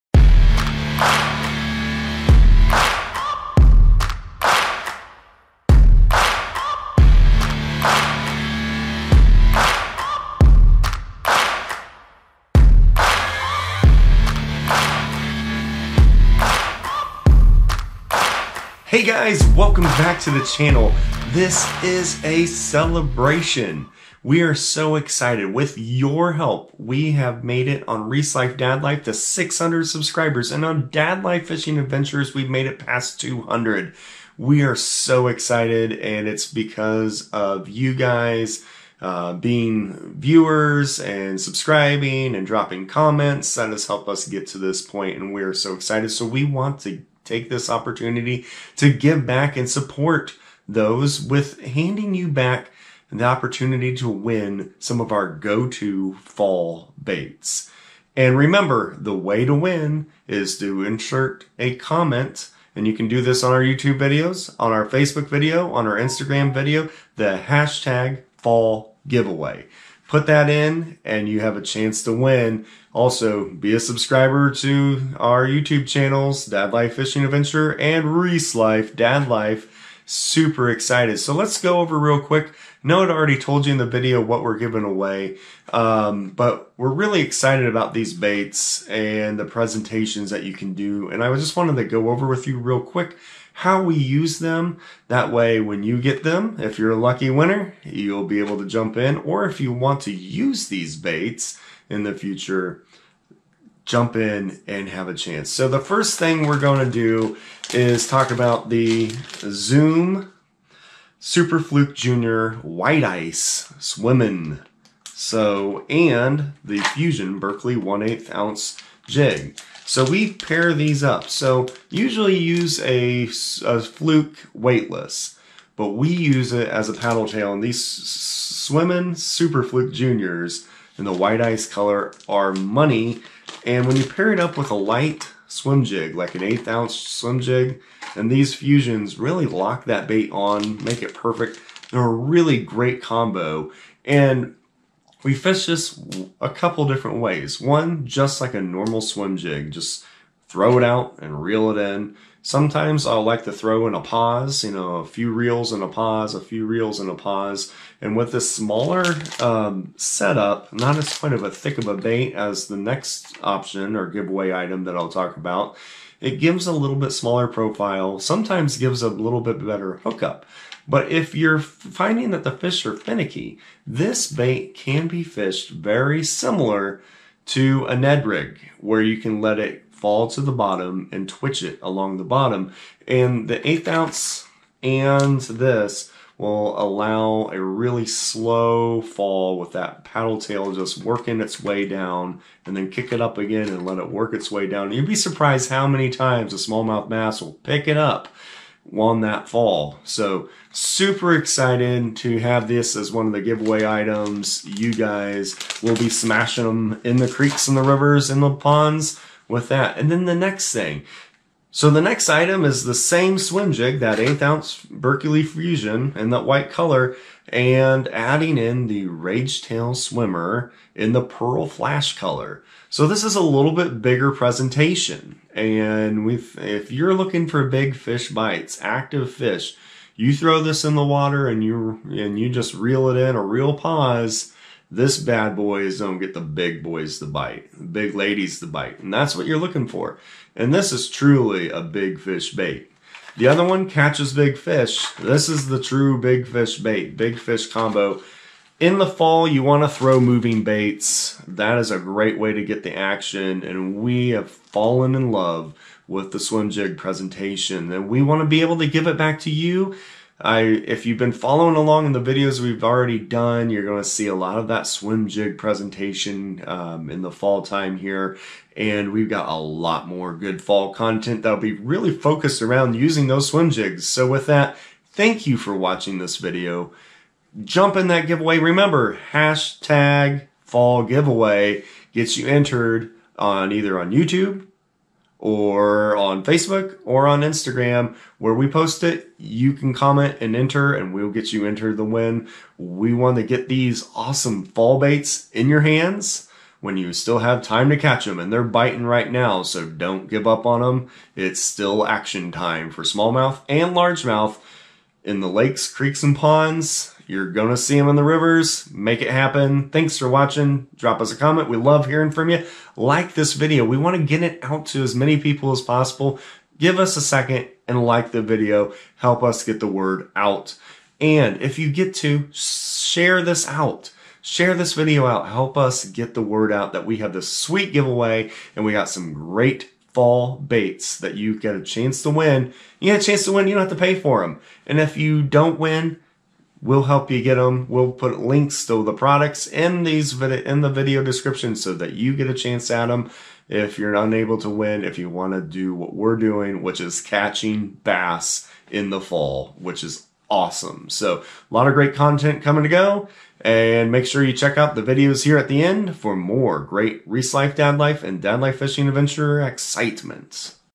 FALL FISHING CELEBRATION GIVEAWAY!!
Enter to win the fallfishing giveaway that we are doing in celebration of Reece Life and Dad Life hitting 600 subscribers and Dad Life Fishing Adventures passing 200 subscribers on YouTube! You can enter to win by commenting fallfishing in your comments! Also, help us get the word out about our channel and this great giveaway by liking the video and sharing it with your friends. You can enter to win here on YouTube, Instagram, or on Facebook!